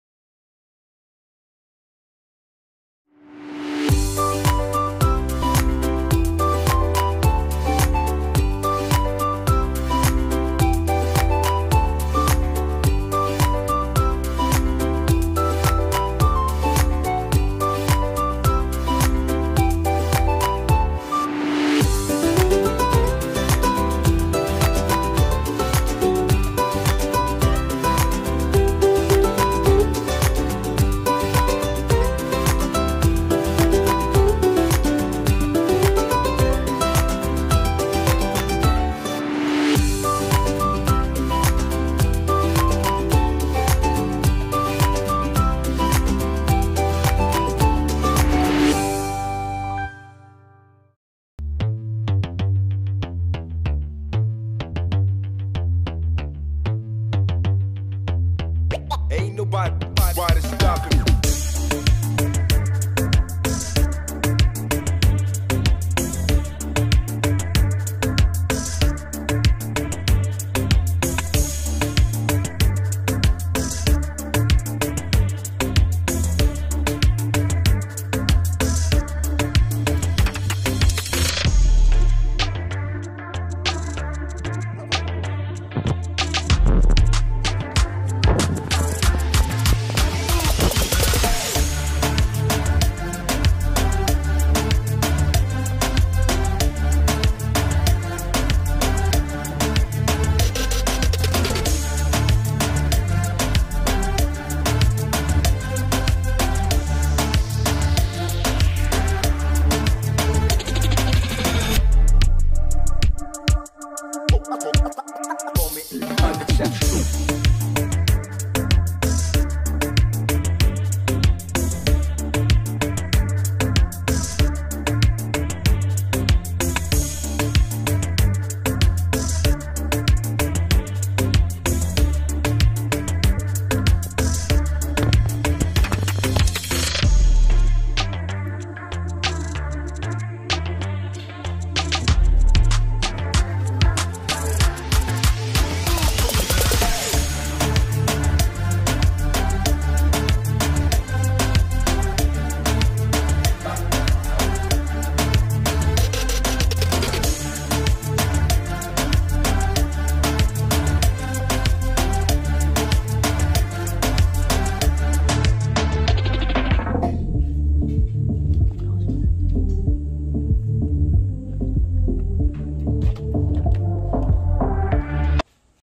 Miniature House for Grade V-EPP
Gumawa kame ng miniature house using recycled materials and things we found in the house. =D